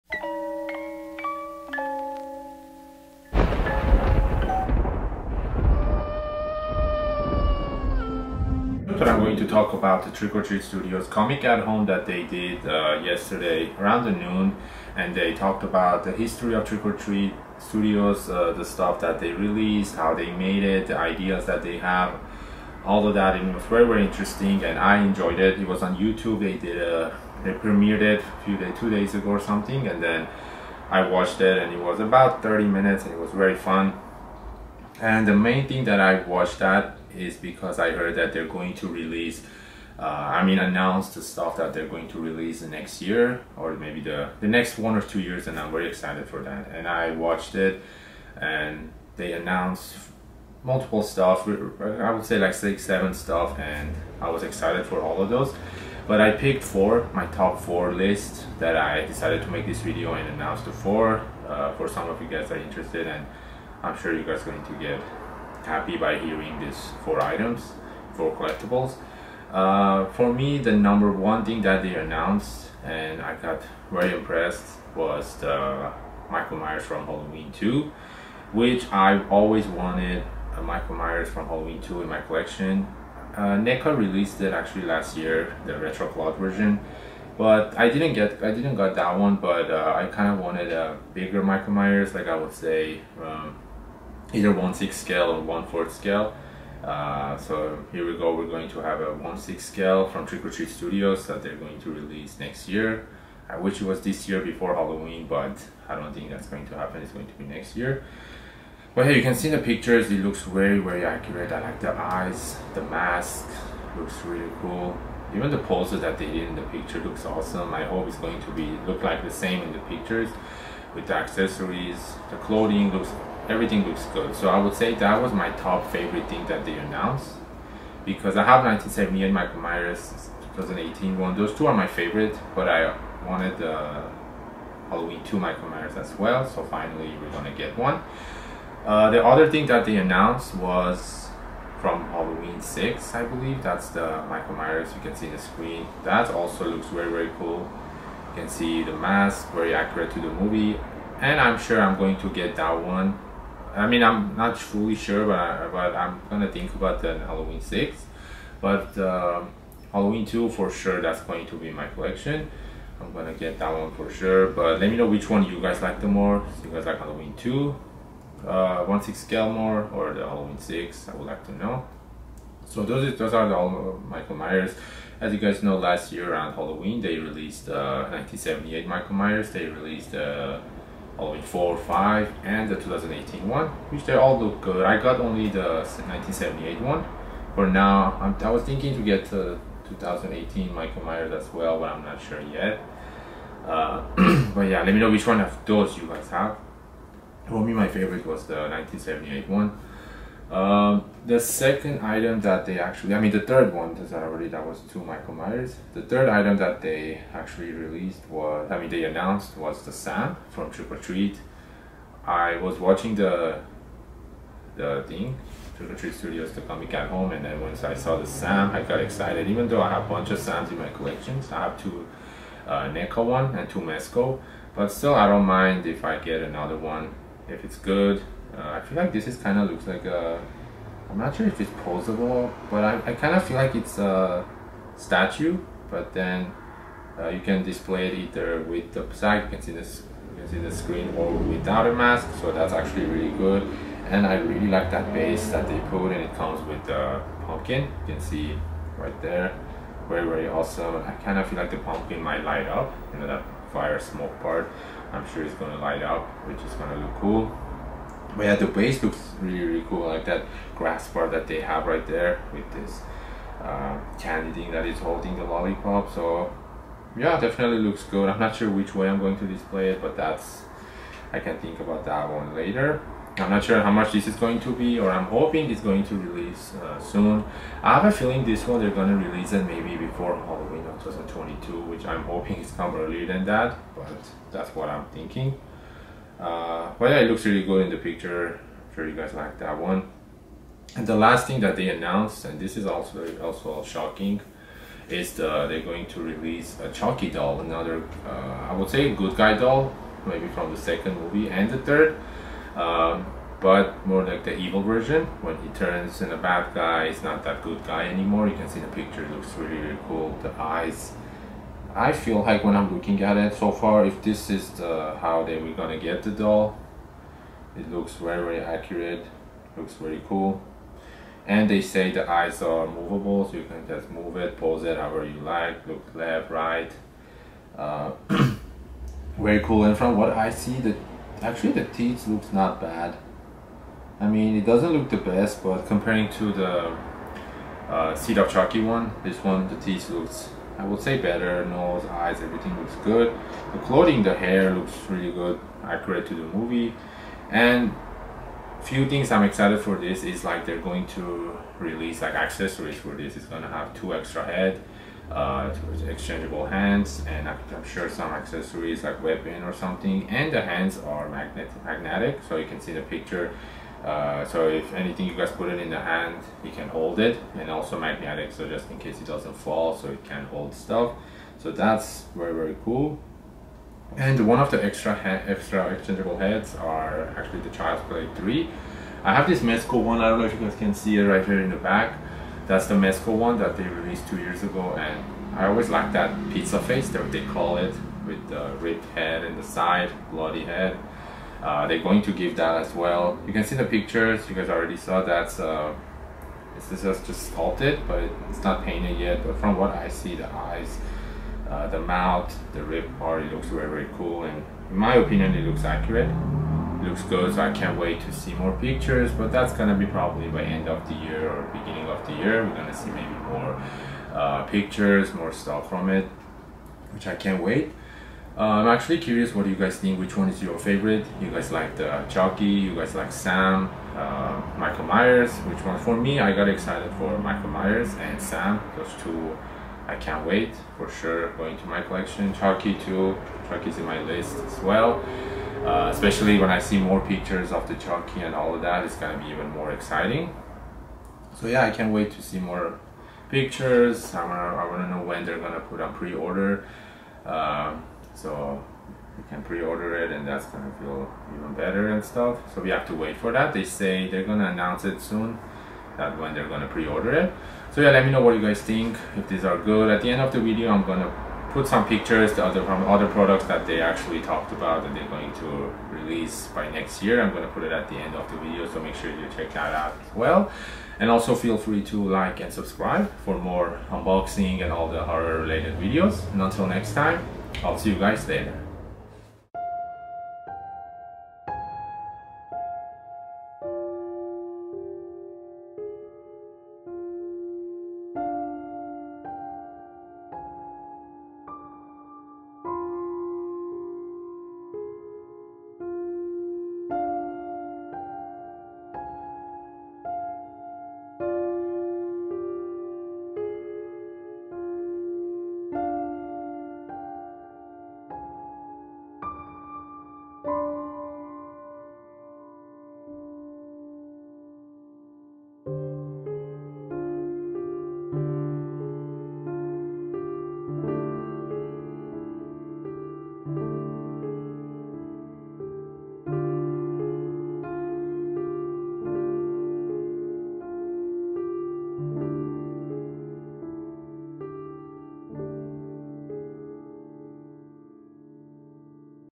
Trick Or Treat Studios Comic Con Product Reveals Preview
Let me know your thoughts about these upcoming trick or treat studios products and let me know which ones you guys like the most!